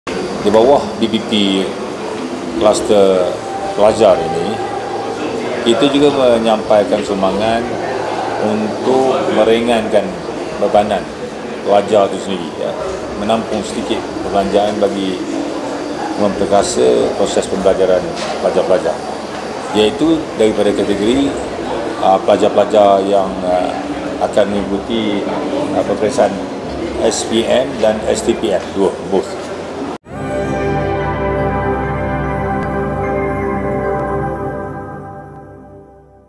KERAJAAN PAHANG PERUNTUK RM2.6 JUTA UNTUK PELAJAR SPM, STPM
|Sedutan Video| KERAJAAN PAHANG PERUNTUK RM2.6 JUTA UNTUK PELAJAR SPM, STPM

“Di bawah Bantuan Prihatin Pahang (BPP) ada kluster untuk pelajar dan kita menyampaikan sumbangan untuk meringankan bebanan pelajar itu sendiri, menampung sedikit perbelanjaan bagi memperkasa proses pembelajaran pelajar-pelajar.-Exco Pertanian, Industri Asas Tani, Bioteknologi dan Pendidikan Negeri Pahang, YB. Dato’ Sri Ir. Haji Mohd Soffi Tan Sri Abd. Razak.